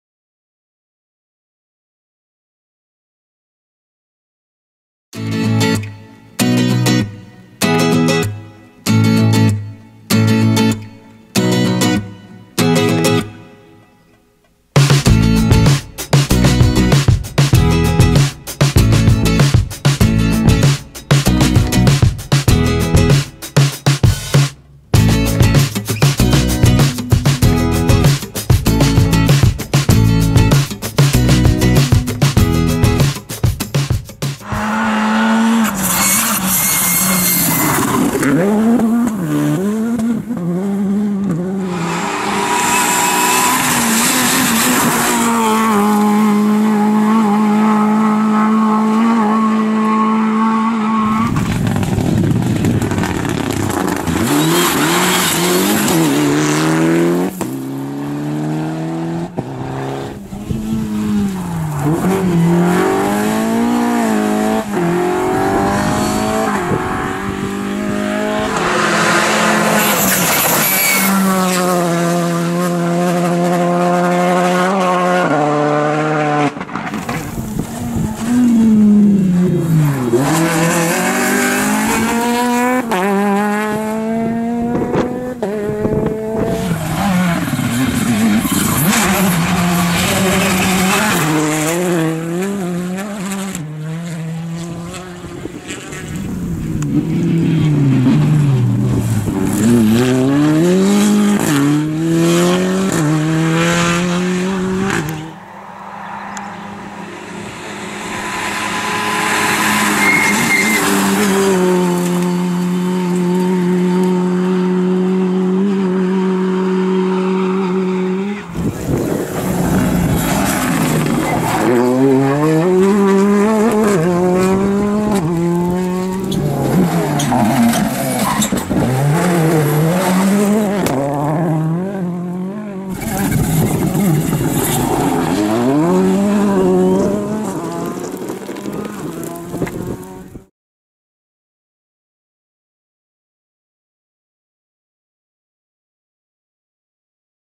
15. Agrotec Petronas Rally Hustopeče 2019
Vinohrady, šotolina, sluníčko, spousta prachu a zaječí kaskadéři! Ve videu z Rally Hustopeče 2019 vystupují dvakrát, zkuste je tam najít.